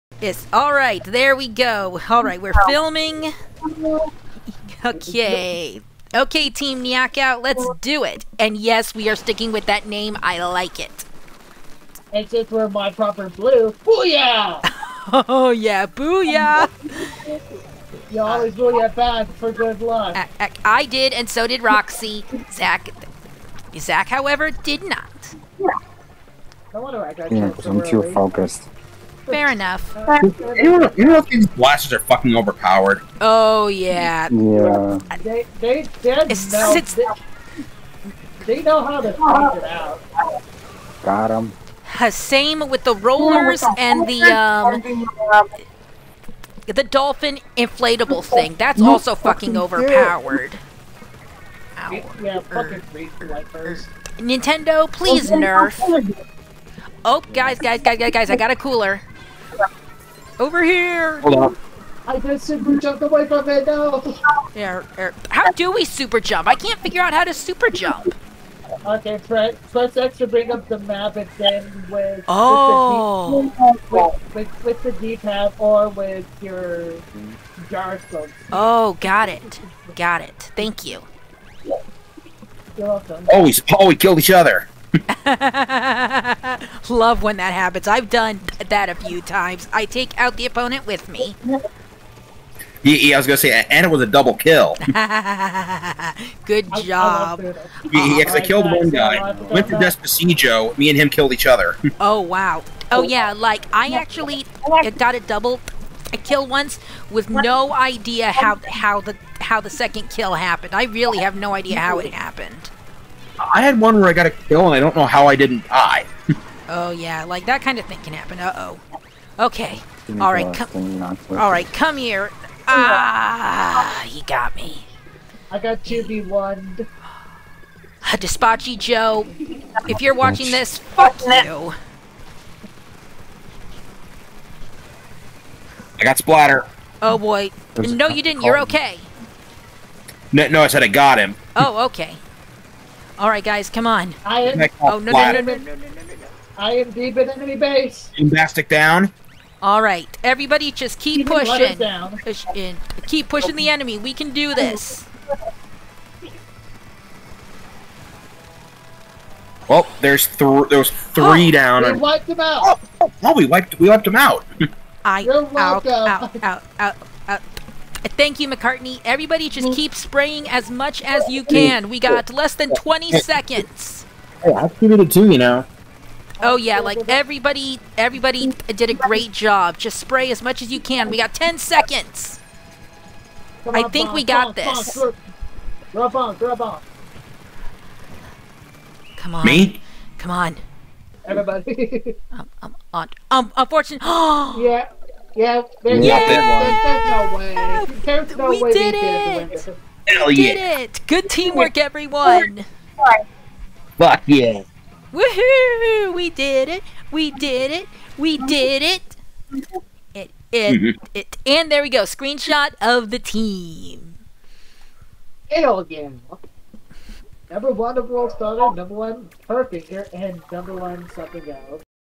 A comeback victory!
Team Nyack-out is finally together and we decided to play some matches.

Here's our come from behind victory